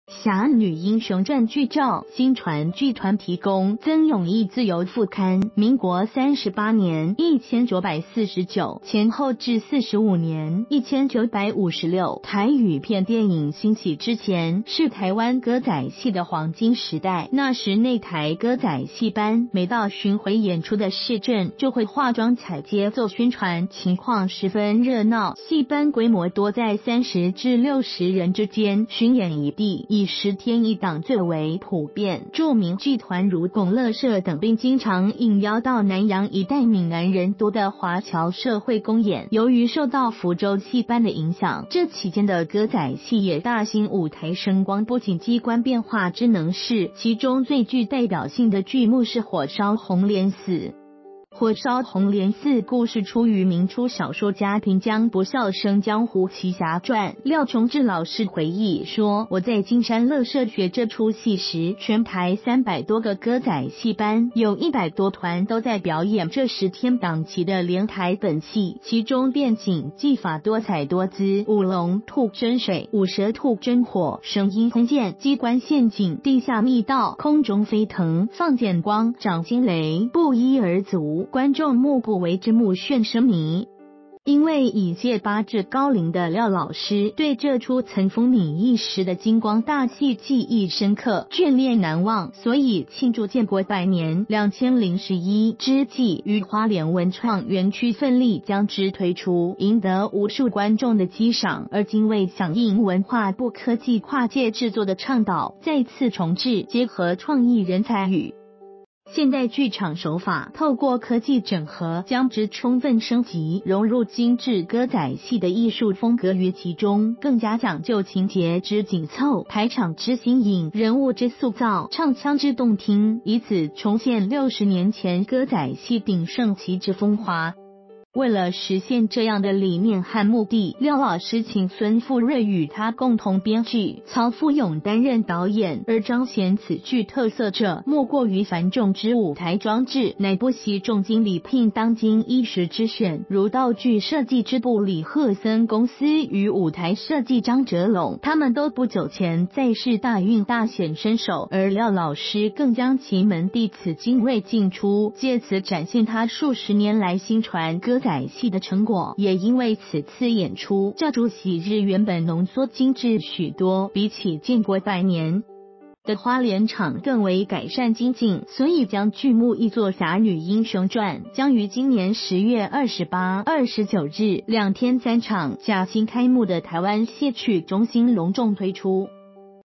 廖瓊枝推新作《俠女英雄傳》
《俠女英雄傳》劇照。（薪傳劇團提供）〔曾永義／自由副刊〕民國38年（1949）前後至45年（1956）台語片電影興起之前，是台灣歌仔戲的黃金時代。那時內台歌仔戲班，每到巡迴演出的市鎮，就會化妝「綵街」做宣傳，情況十分熱鬧。戲班規模多在三十至六十人之間，巡演一地，以十天一檔最為普遍。著名劇團如「拱樂社」等並經常應邀到南洋一帶閩南人多的華僑社會公演。由於受到福州戲班的影響，這期間的歌仔戲也大行舞台聲光布景機關變化之能事，其中最具代表性的劇目是《火燒紅蓮寺》。

發表評論評論如下。